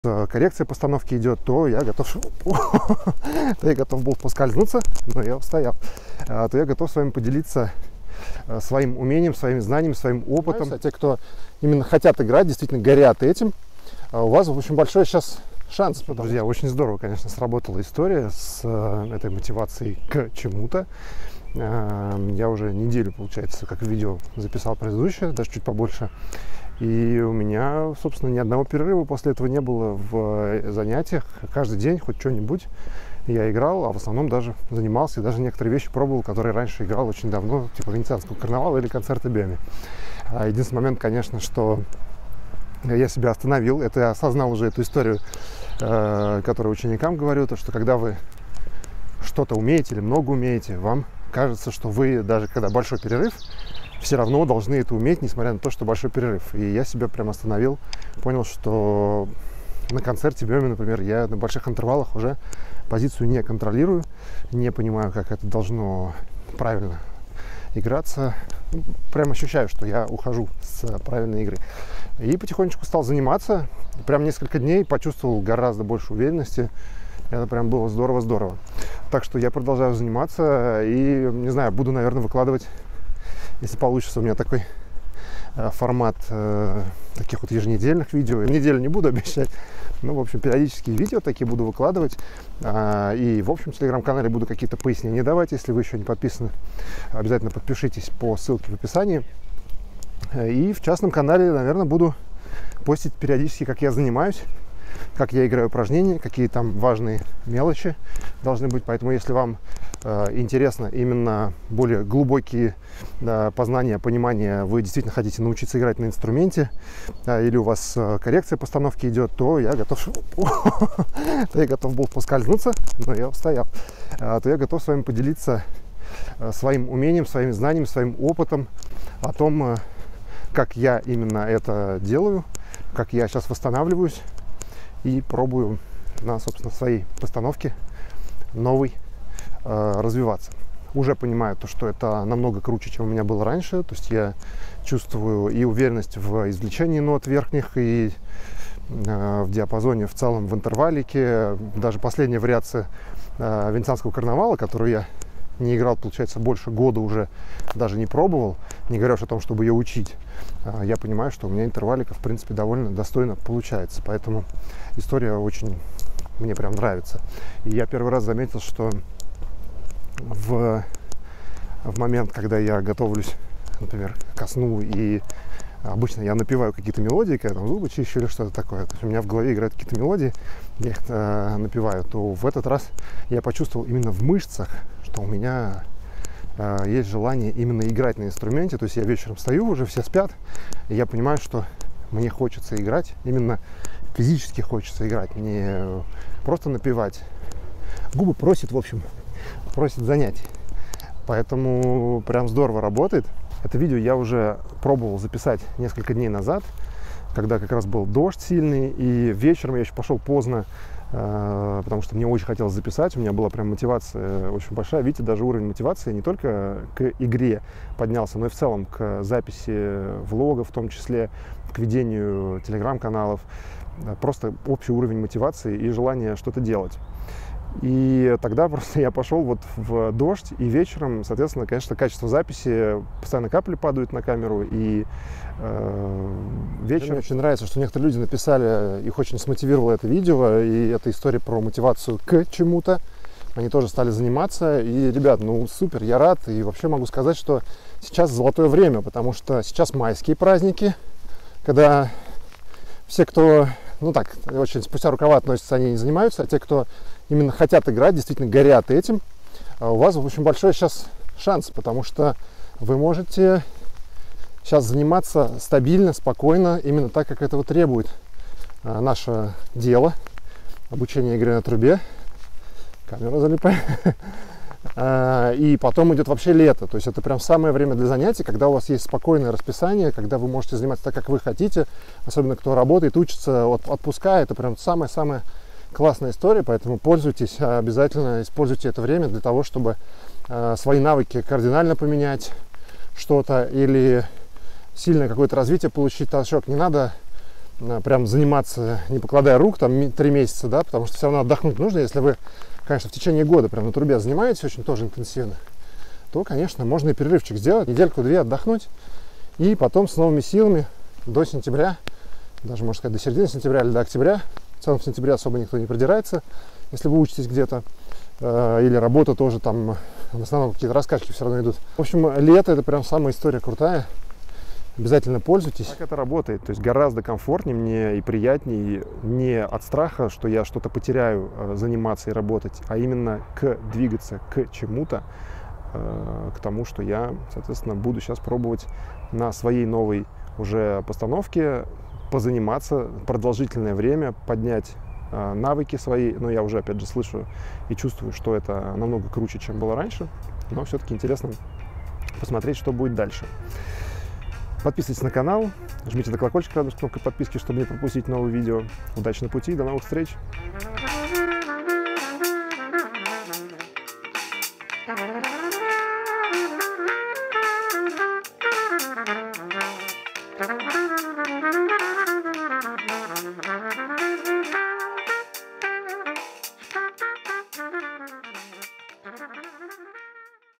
ДНЕВНИК Трубача_2. Фатальная ОШИБКА при обучении игре на трубе
Сегодня  расскажу на своем примере про одну очень частую ошибку при восстановлении после длительного перерыва и при коррекции постановки. В свое время это стало для меня фатальной ошибкой в обучении, так как я постоянно то ловил волну, то больно падал с нее.